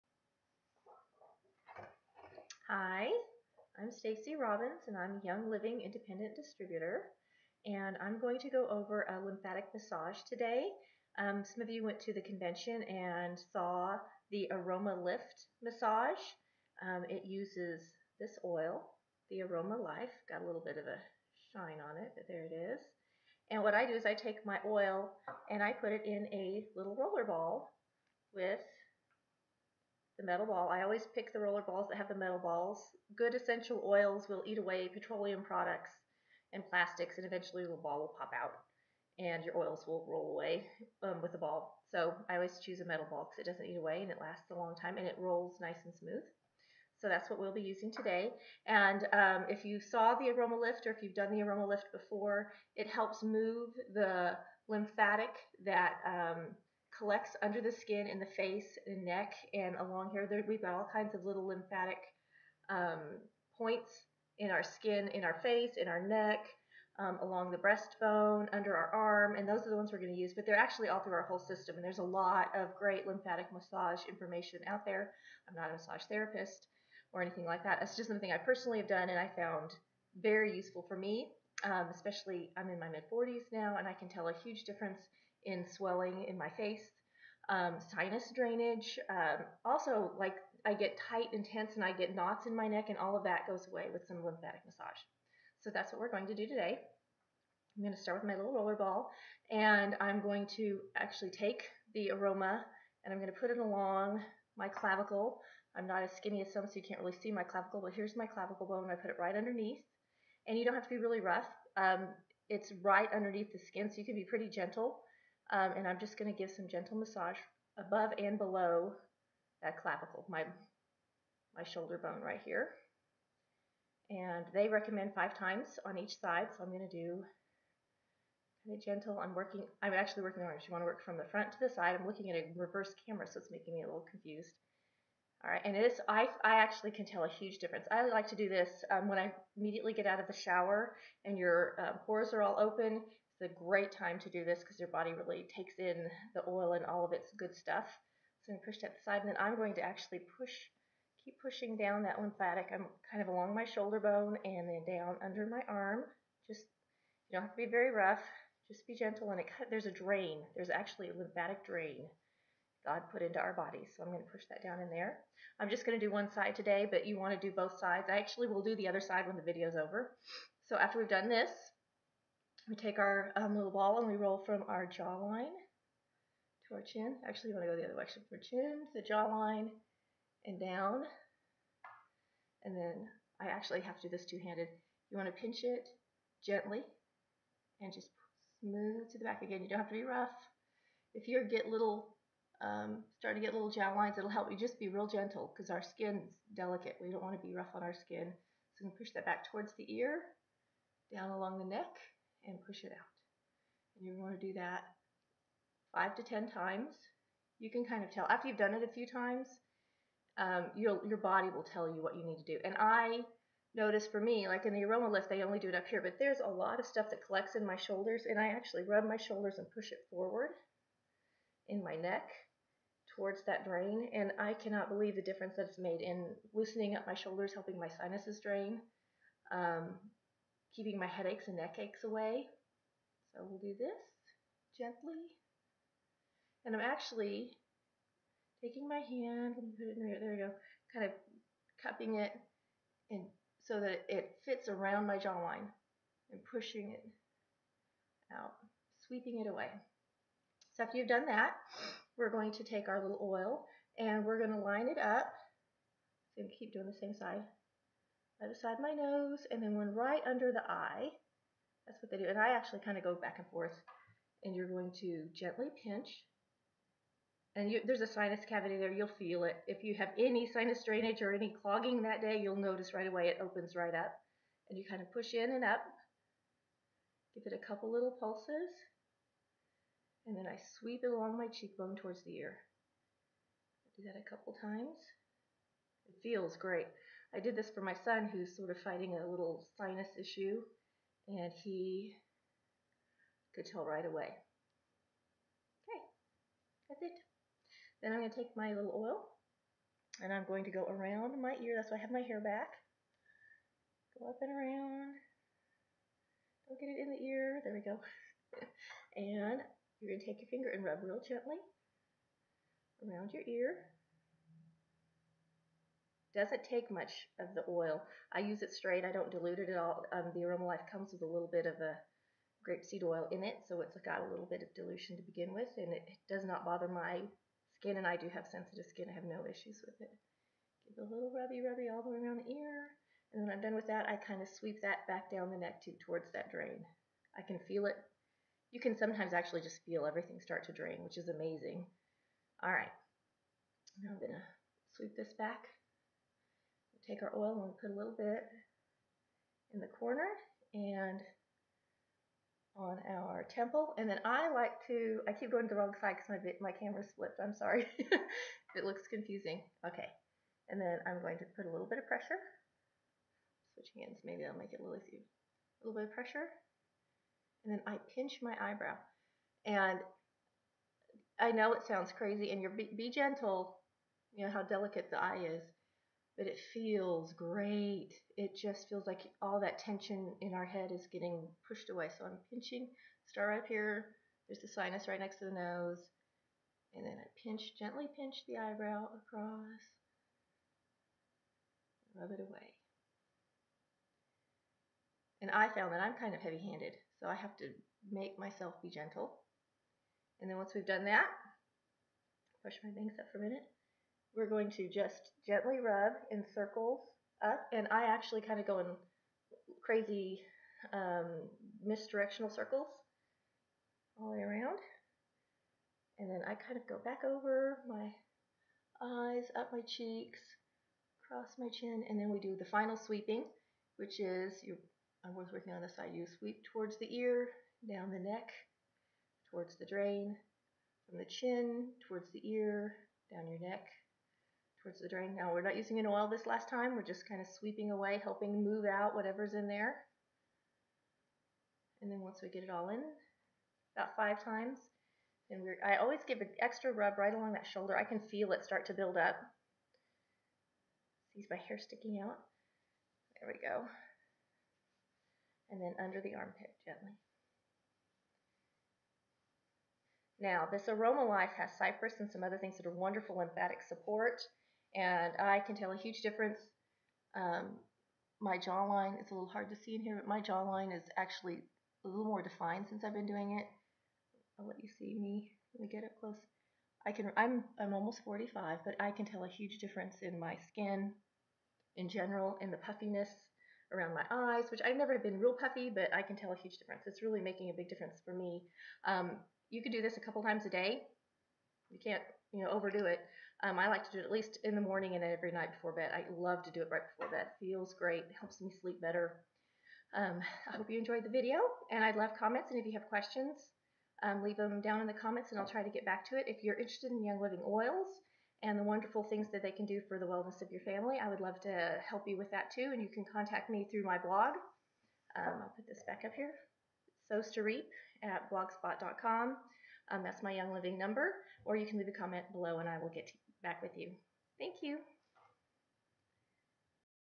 Lymphatic Massage for Face and Neck
This massage  uses Aroma Life Essential Oil from Young Living to support the movement of lymph stored under the skin.  It helps reduce puffiness in your face, look brighter, and more awake.
Young Living Independent Distributor #1954431
Want to join Young Living and live above the wellness line?